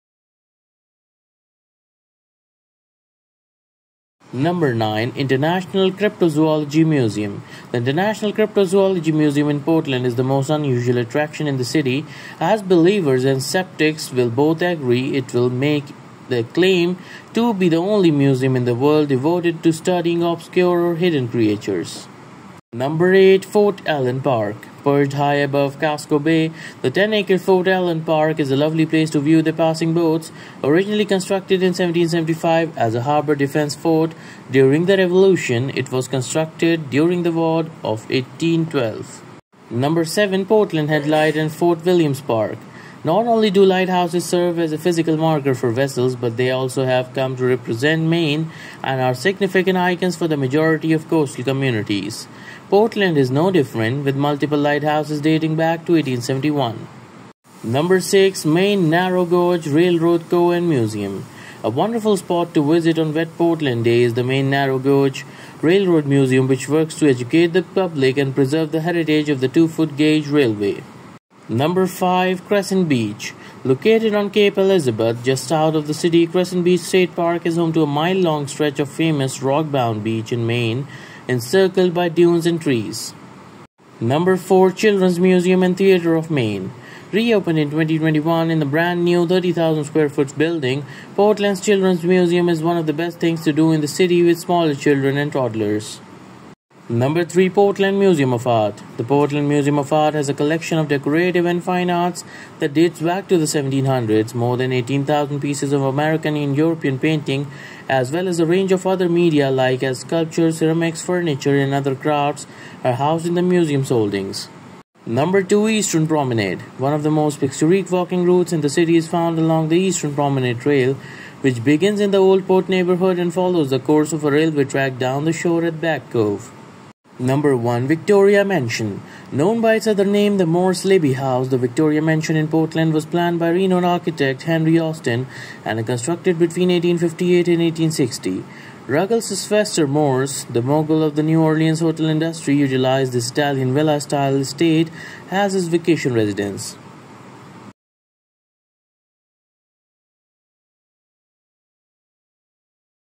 Best Places in Portland, Maine | USA - English
We spent a long time in finding a list of top 10 best places to visit in Portland. It includes all of the different entertainment options including things to do in Portland. There are hundreds of other locations here, but we have selected the famous places in Portland for you, so you can enjoy your visit. There are many beautiful places in Portland which people prefer, it depends on their own interest.